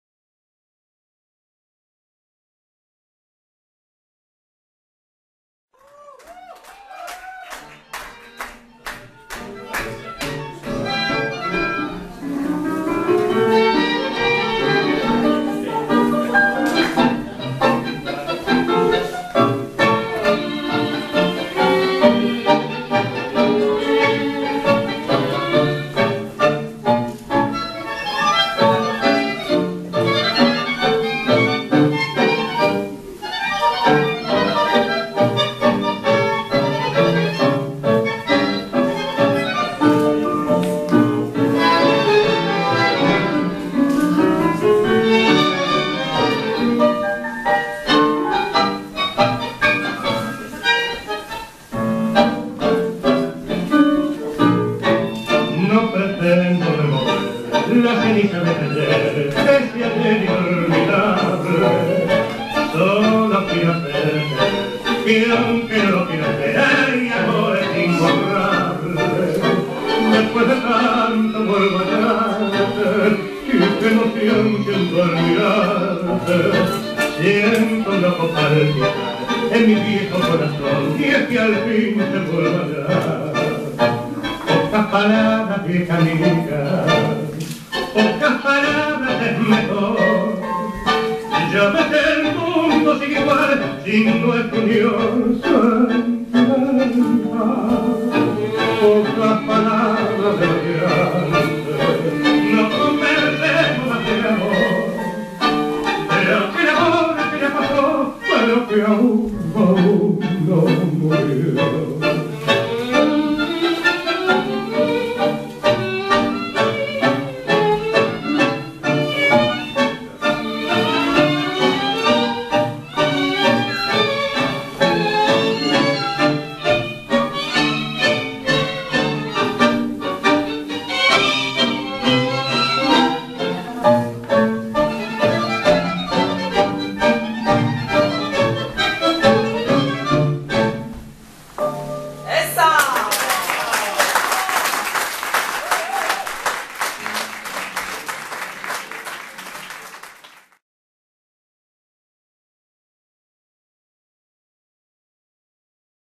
Luiza Almiron, Gonzalo Alonso & Mariel Robles, Marcelo Almiron, teacher's tango show Pacoltowo 2014)
Luiza Almiron, Gonzalo Alonso & Mariel Robles & Marcelo Almiron, teacher’s tango show. Pacoltowo Palace Milonga (Tango Holiday in Pacoltowo Palace, Poland, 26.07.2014).